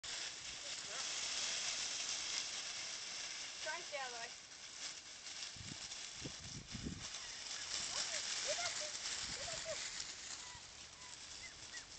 Dogs playing in a pile of leaves
Chloe and the litter in Autumn